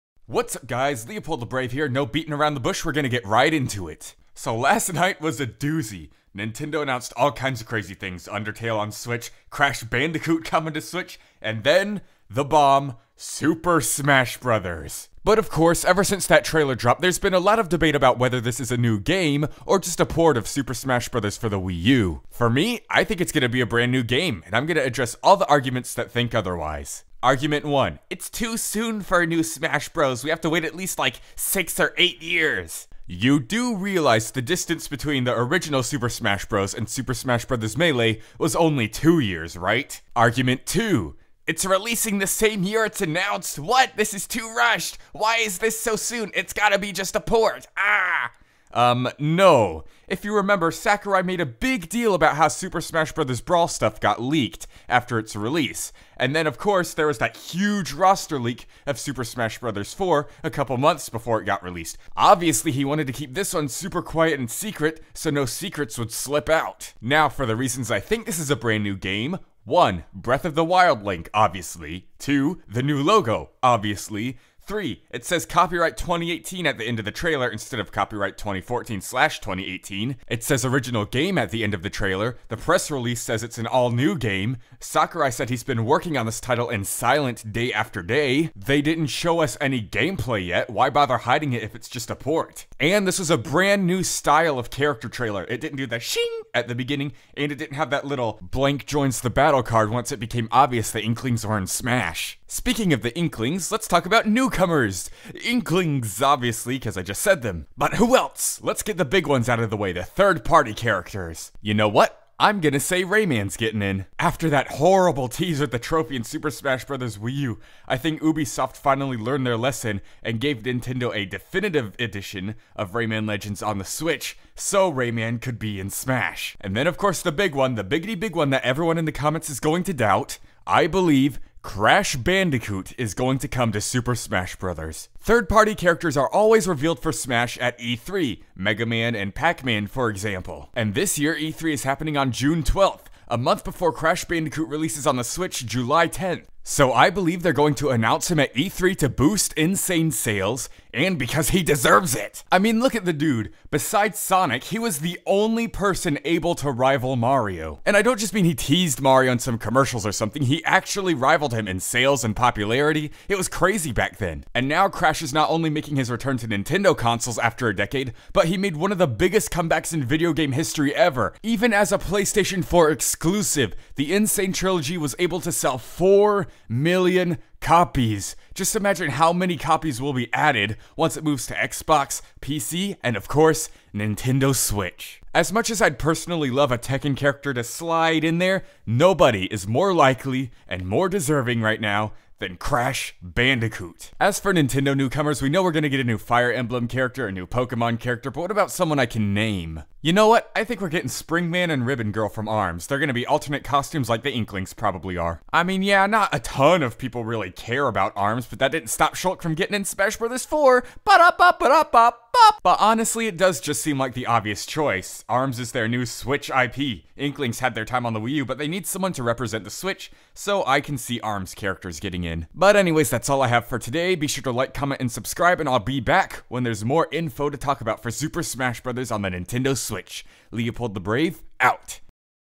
SUPER SMASH BROS. SWITCH DISCUSSION - New or port? Why so soon? NEWCOMERS!?
Who do you think it making the cut!?